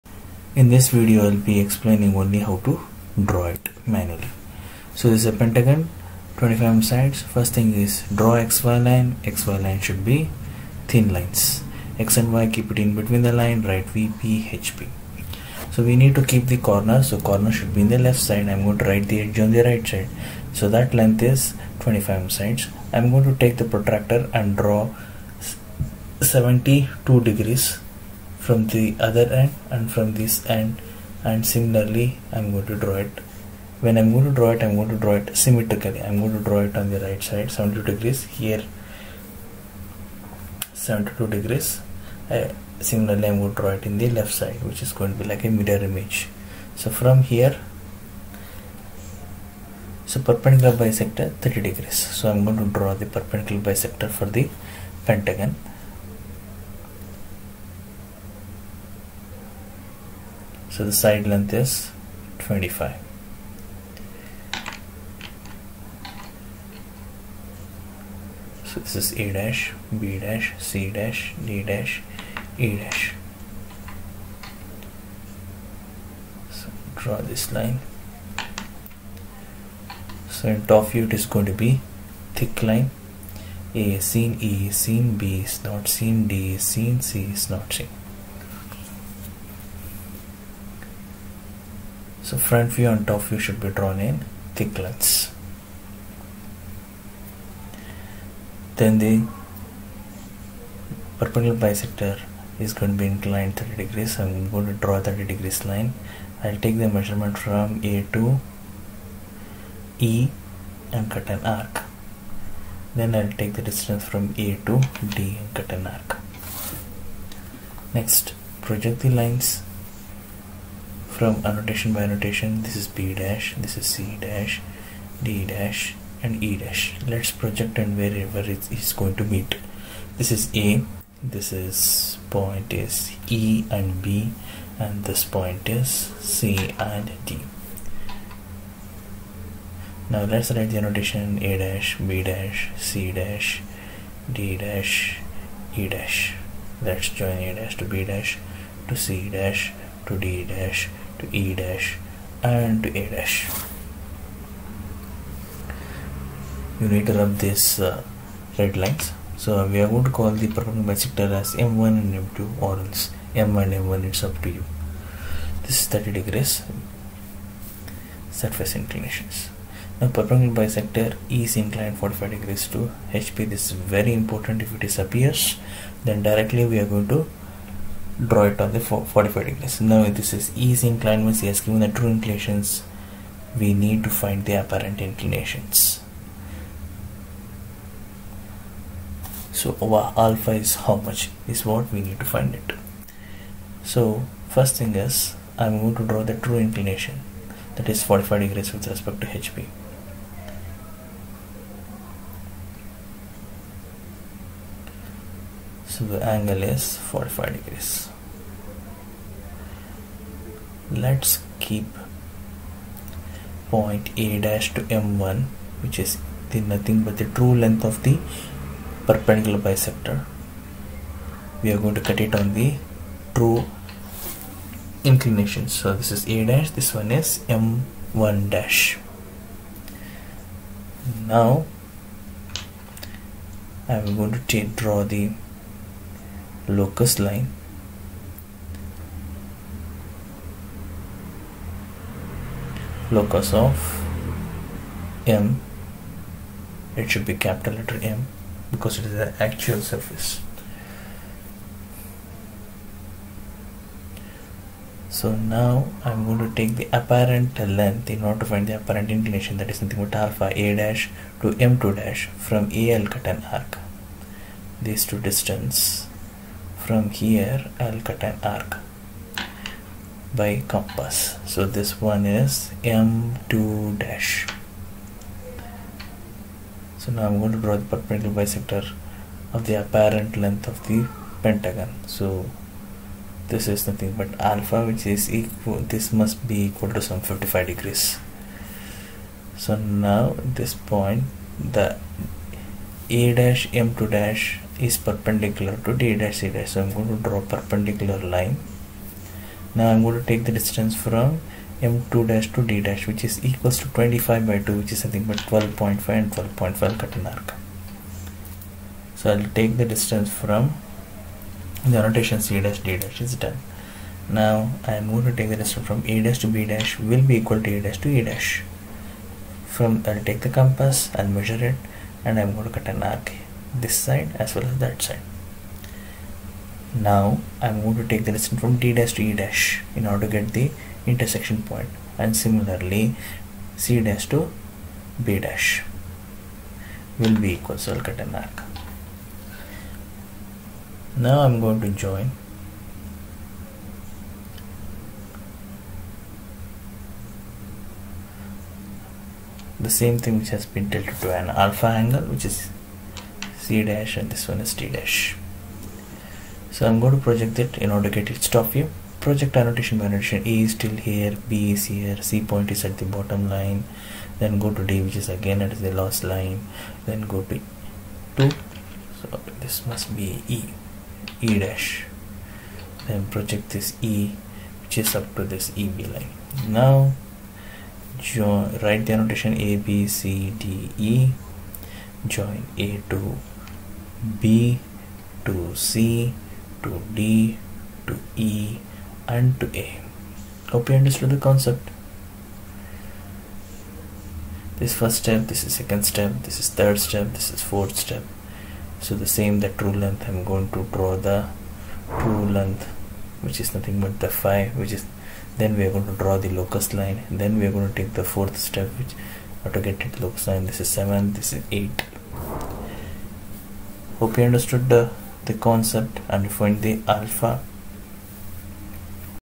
EGDL Q31 | MANUAL SKETCHING | PENTAGON LAMINA| VP | CORNER | PERPENDICULAR BISECTOR| ALPHA | PLANES
In this video, you will be learning how to to draw projection of planes problem for the given question.

A pentagonal lamina having edges 25mm is placed on one of its corners on VP such that the surface makes an angle 30° with VP and perpendicular bisector of the edge passing through the corner on which the lamina rests is inclined at 45° to HP. Draw the top and front views of the lamina.